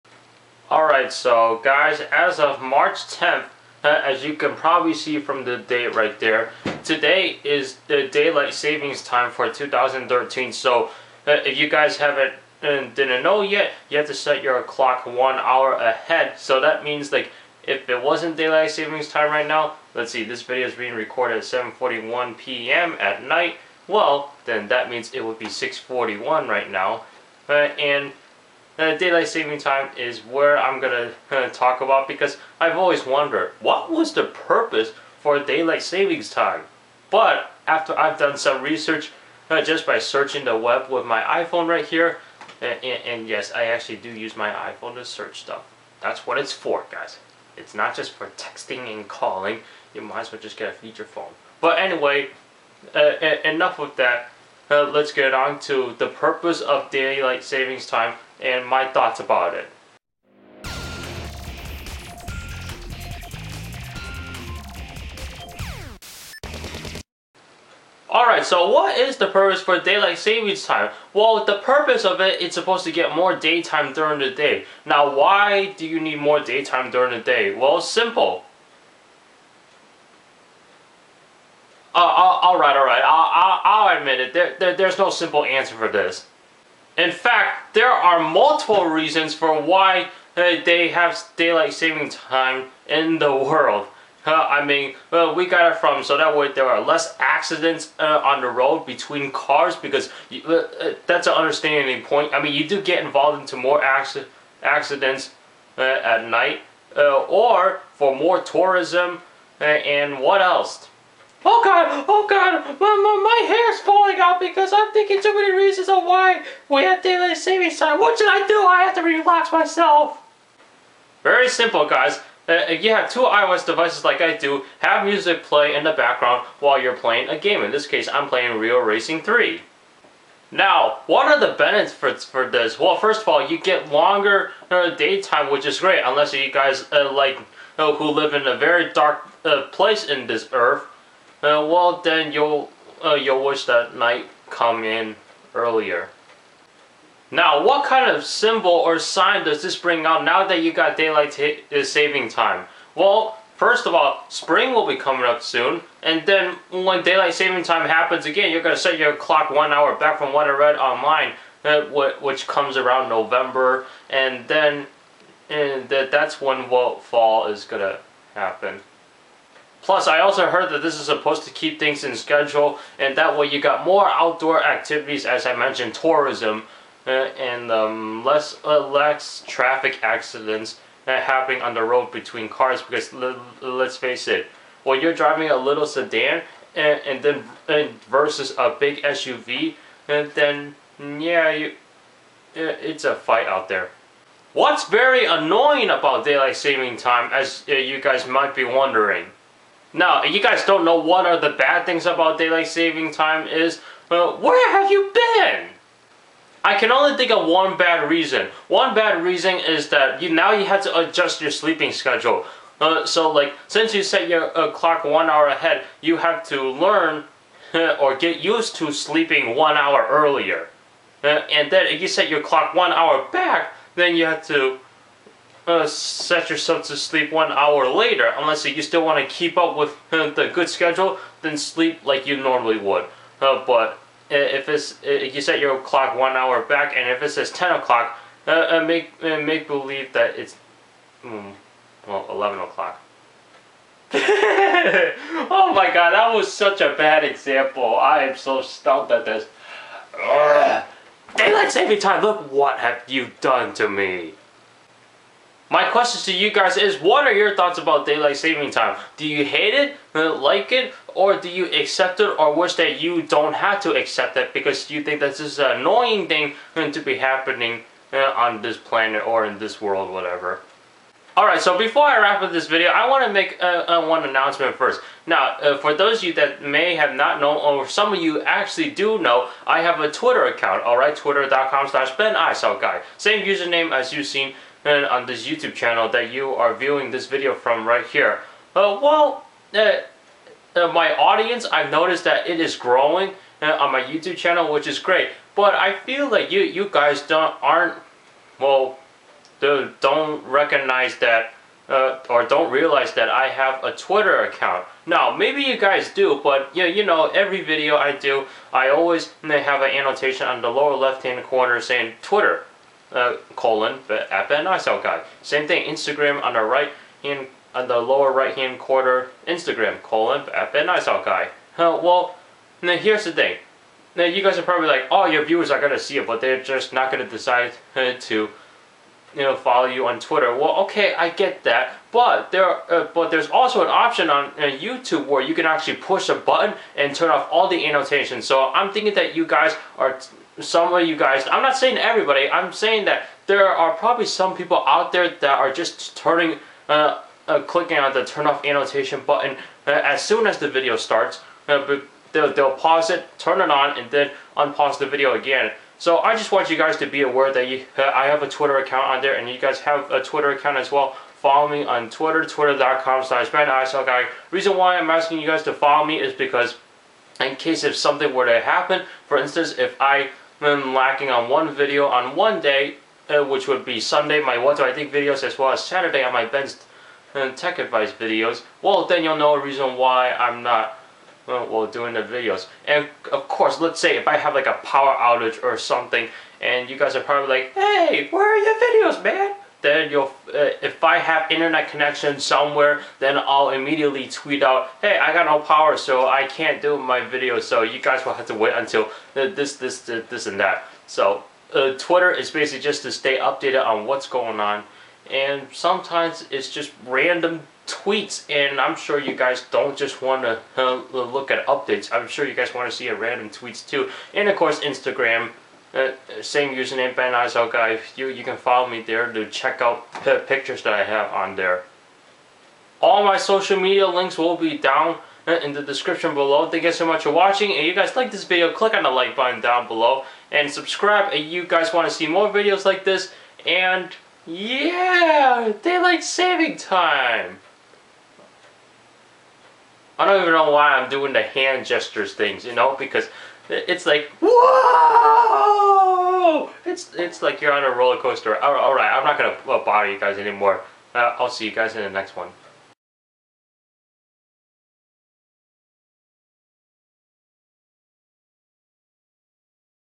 What do I think? #48 Daylight Savings Time
Have you guys ever wondered what daylight savings time is for? What's the purpose for daylight savings time? What are the benefits for this? After doing some research on this, there are reasons why we have this. As I mention the reasons for this, I discuss my thoughts on daylight savings time.